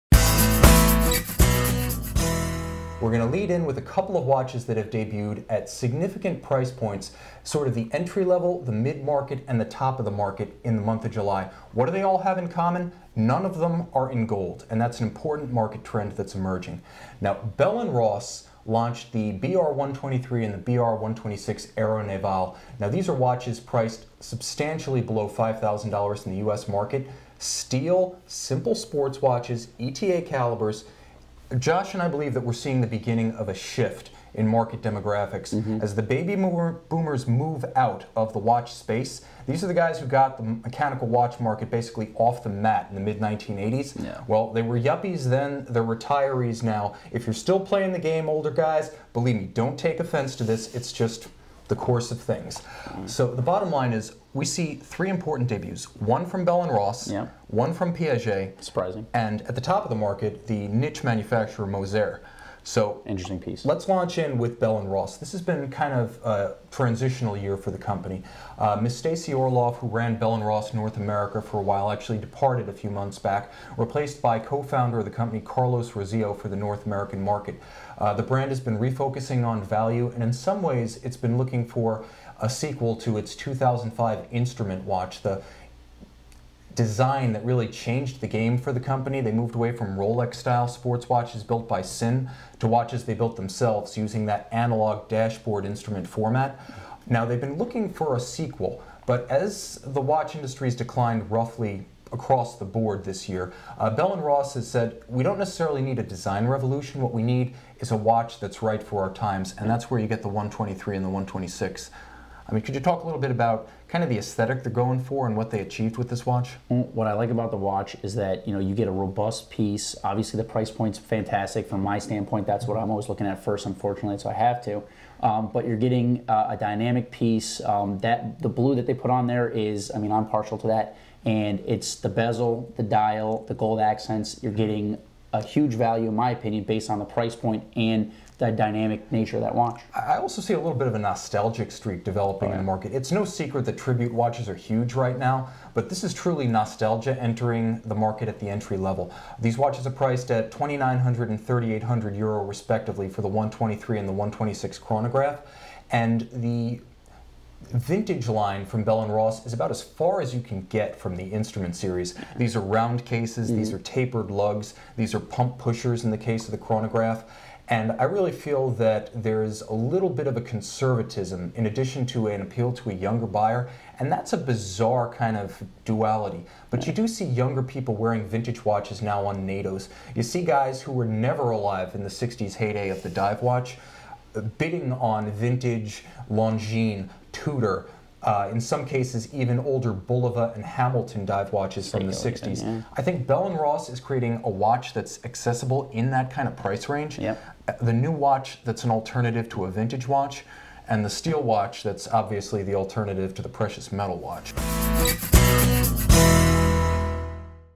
WuW Live Short: New Watches for July
This Week in Watches debuts on watchuwant's YouTube channel with a bit of catching up to do. In this highlight from our July 22 live broadcast, thewatchbox.com Watch Specialist/Media Director and Lead Watch Buyer Joshua Thanos consider several new watch debuts  from **the entirety of July** that share a common thread; none of them are gold, platinum, or precious metal. As baby boomers leave the market and economic uncertainty looms, many watch manufacturers at all levels of the market are embracing steel and titanium luxury watch cases.